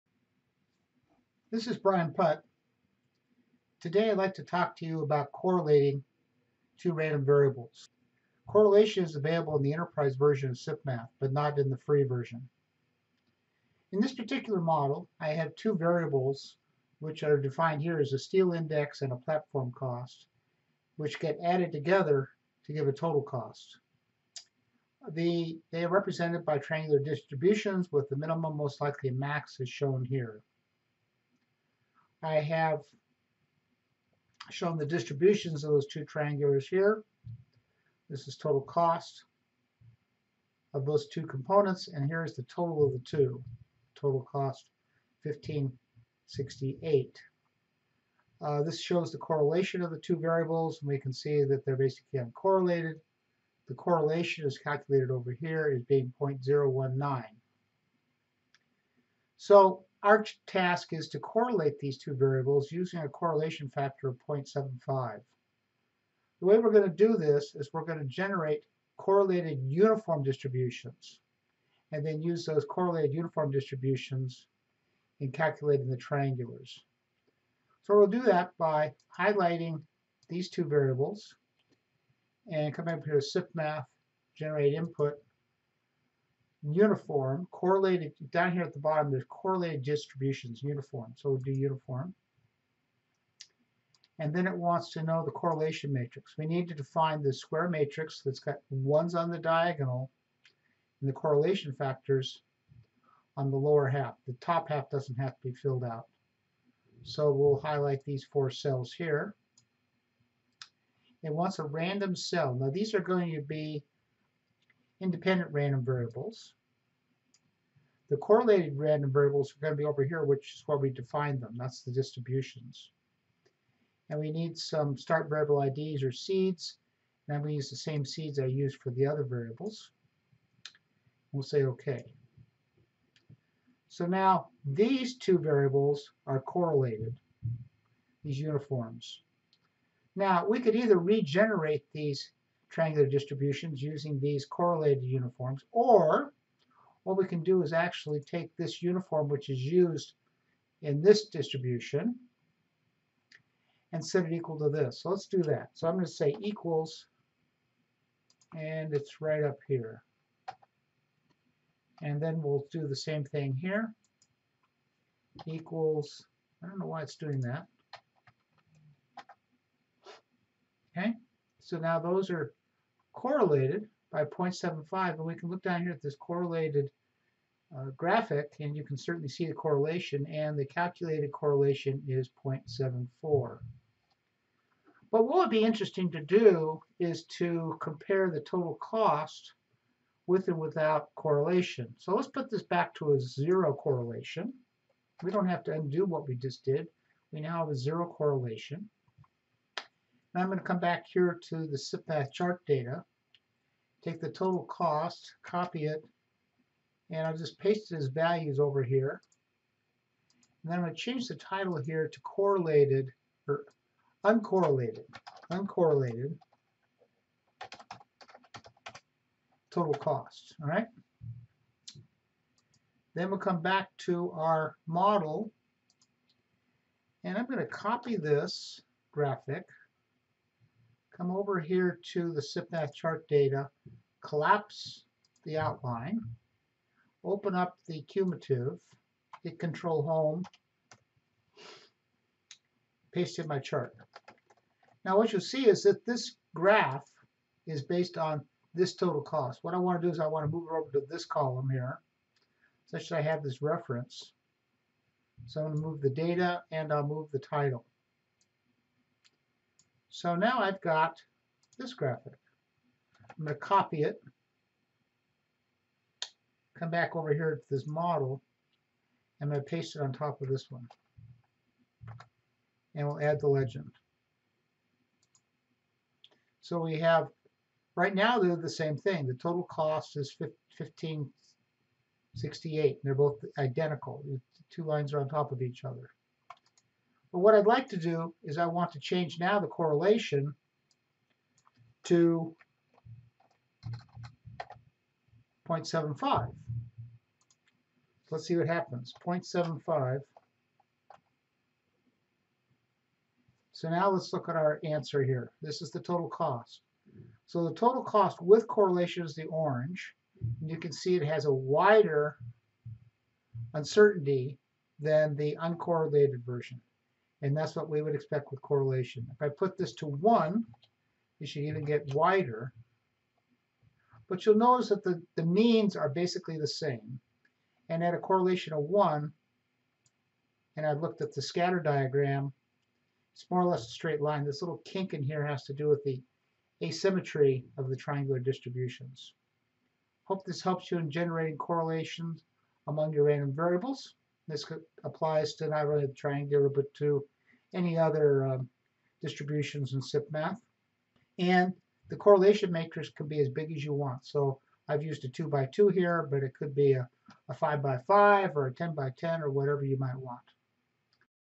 Correlating Distributions in SIPmath Enterprise Tools
Brian Putt adds two random variables in the SIPmath Enterprise Tools, then compares the distributions with and without correlation.